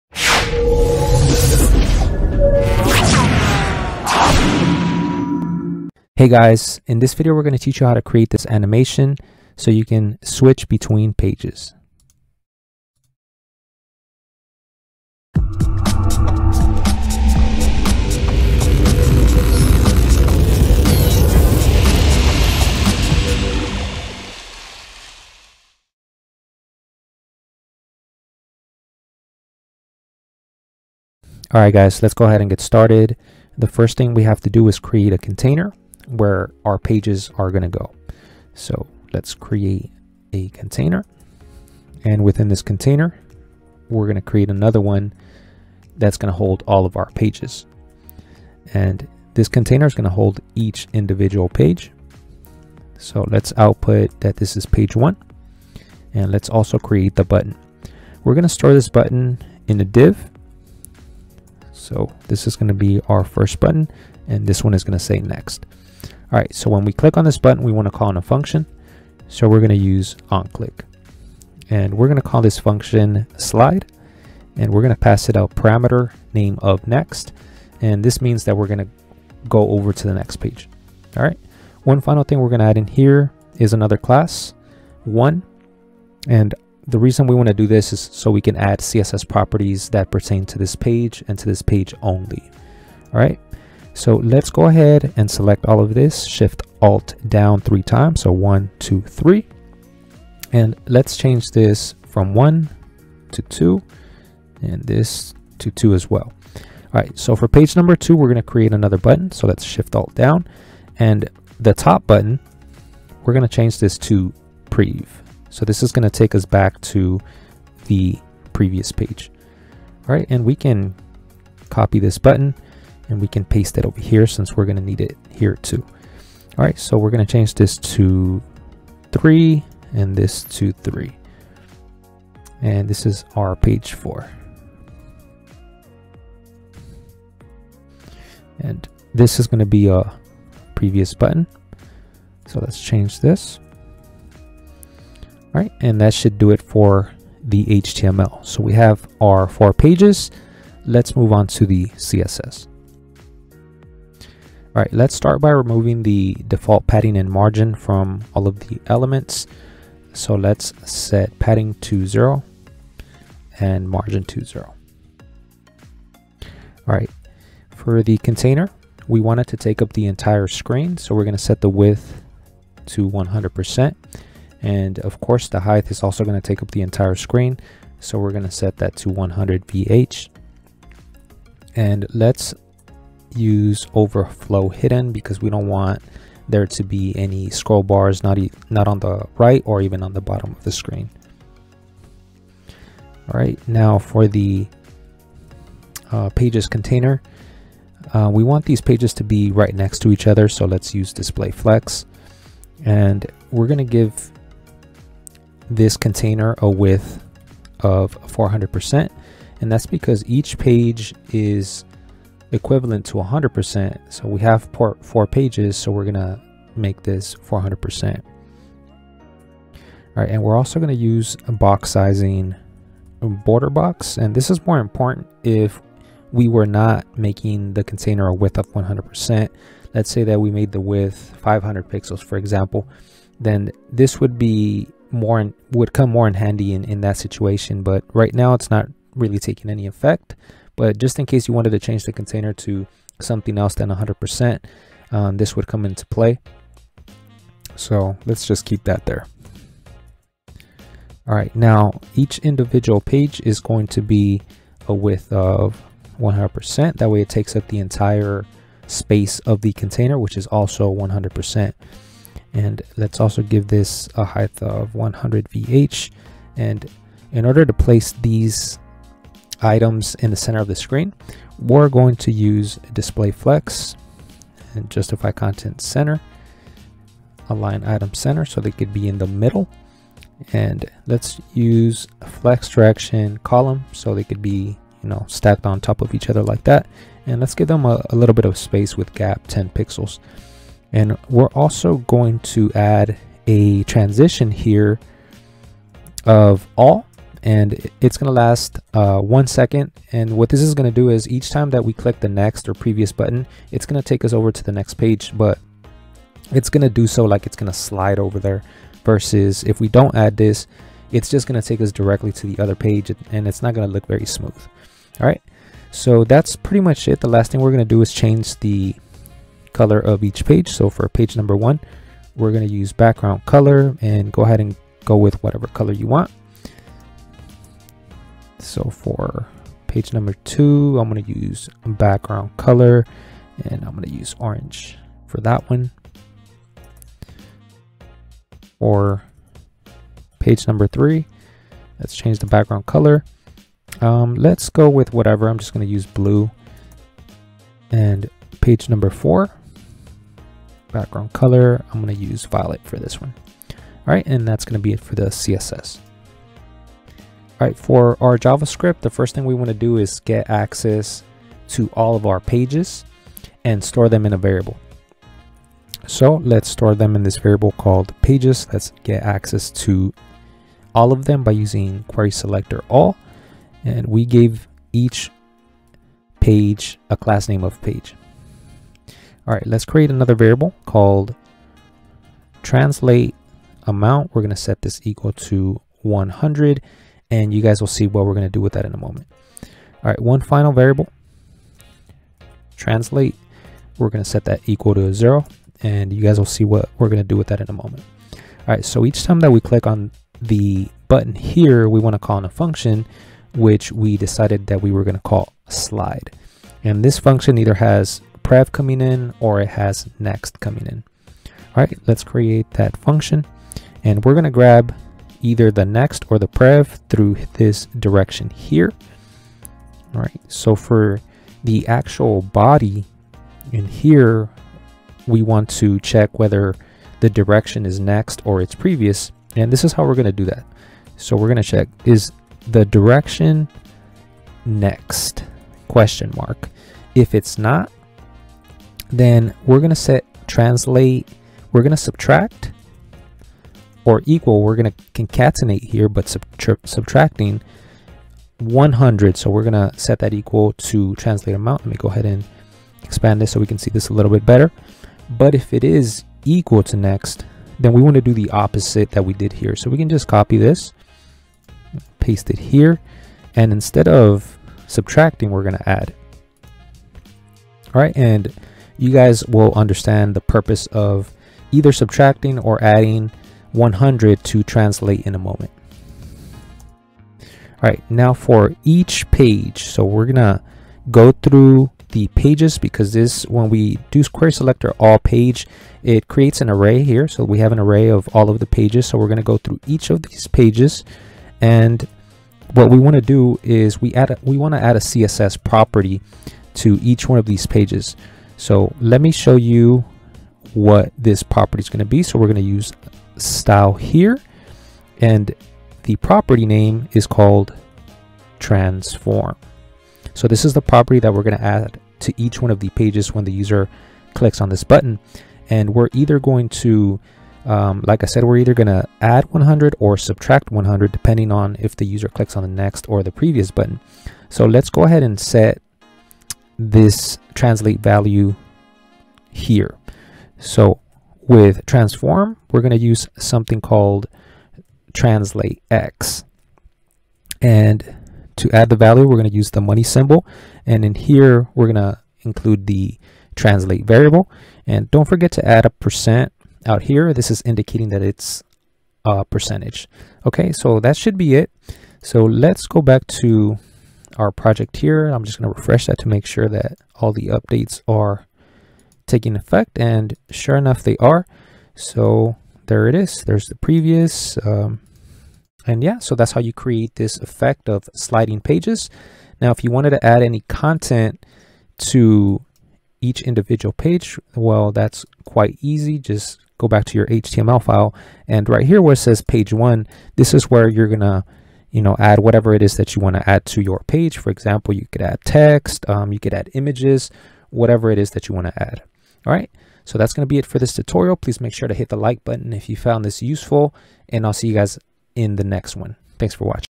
HTML, CSS & JavaScript - How to Slide Between "Pages"
In this tutorial, you'll learn how to slide between pages